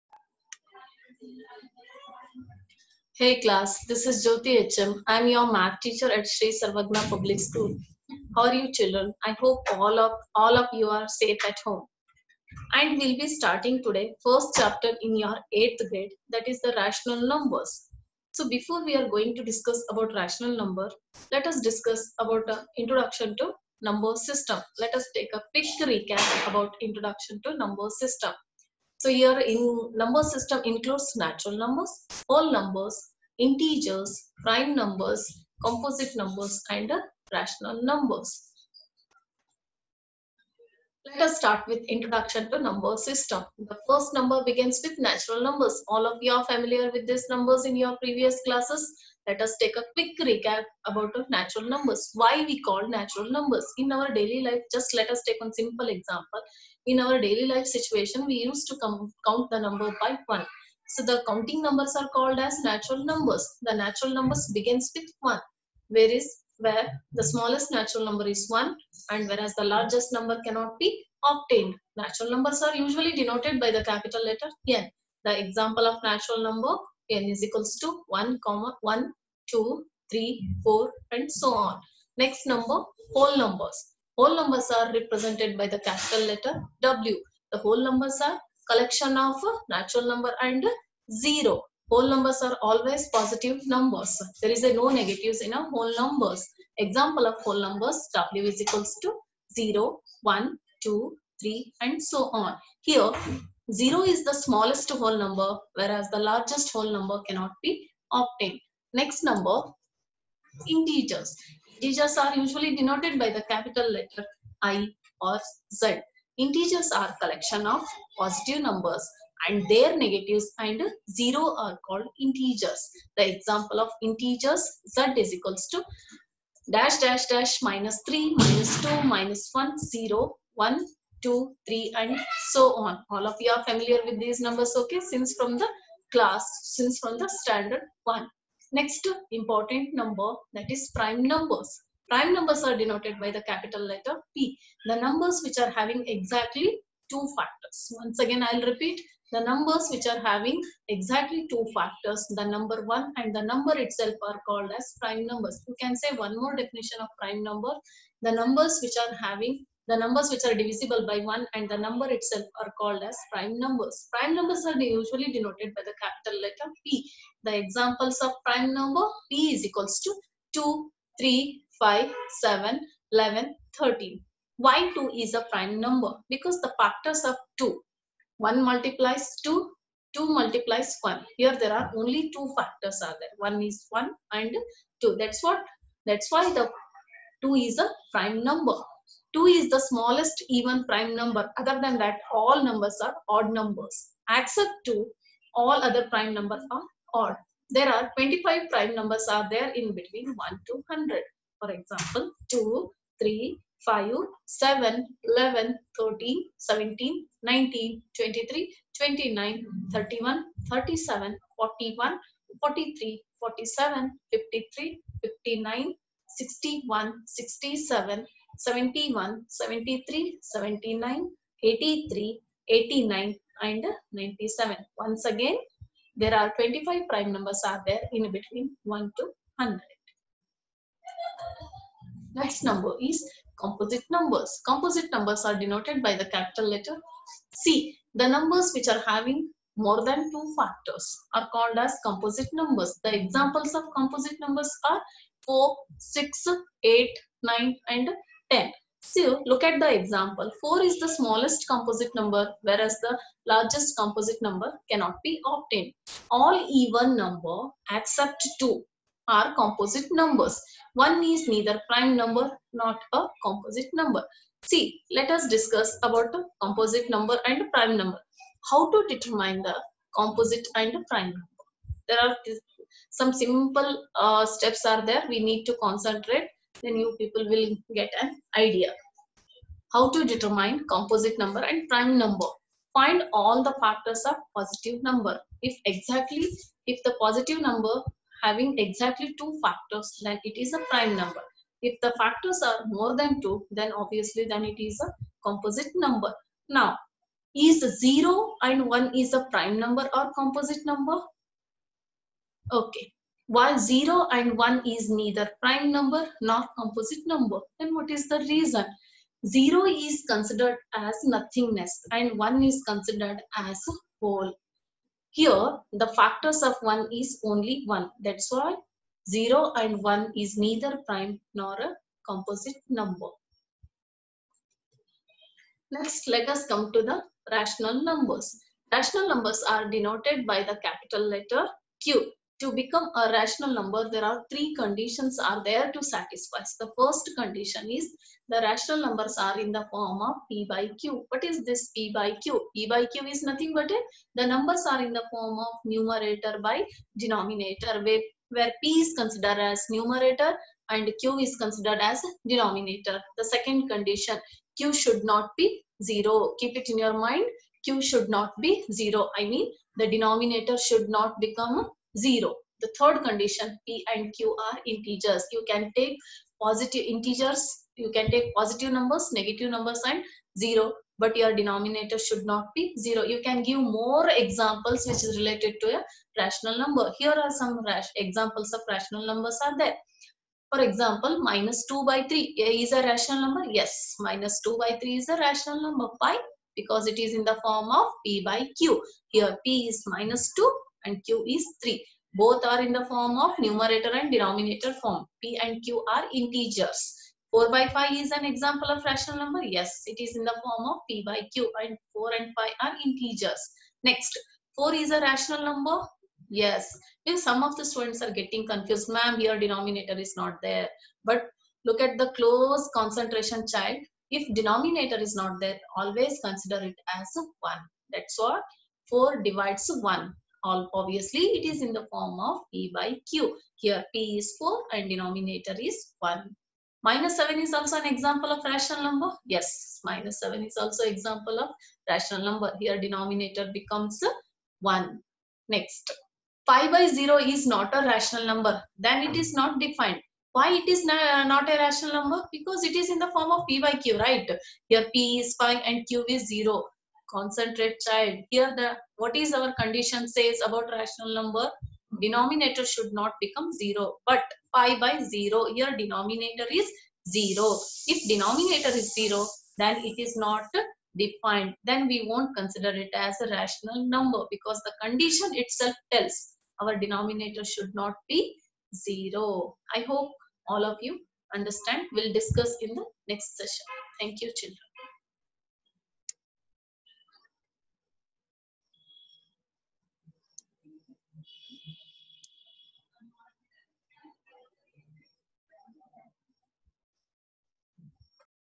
Mathematics ClassVIII Rational Numbers Part1| Sri Sarvajna Public School
This video contains the basics of Rational Numbers